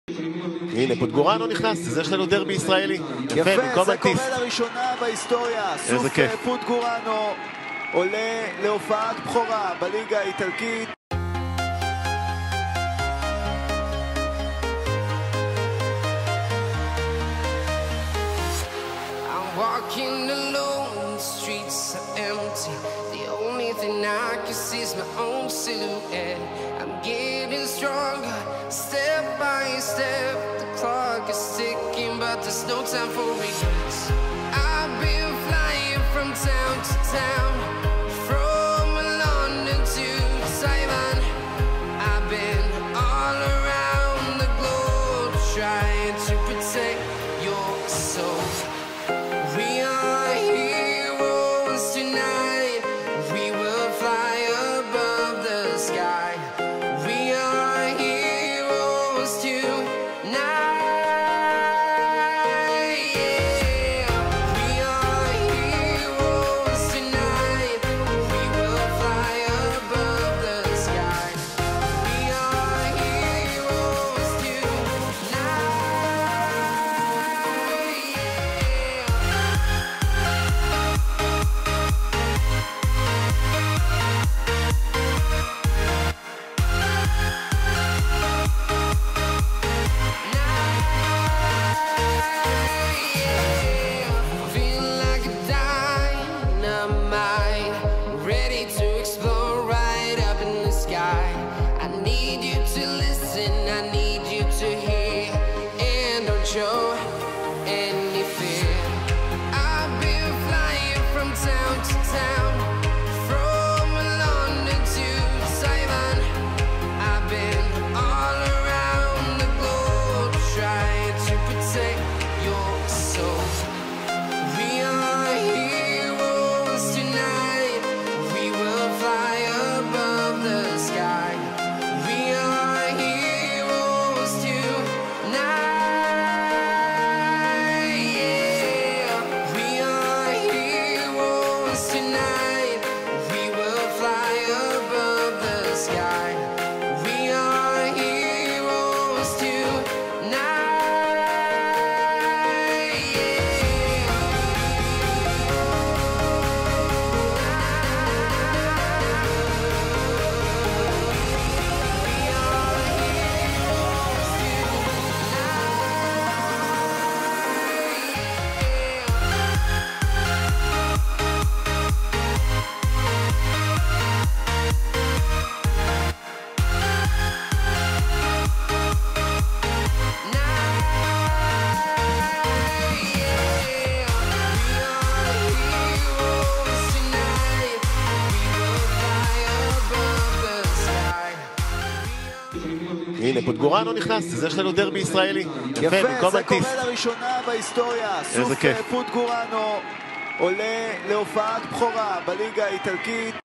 המסע של סוף פודגוראנו (בן 19) להופעת בכורה בליגה האיטלקית במדי ספציה 2021 - SUF PODGOREANU
צילום ועריכה: יוסטאר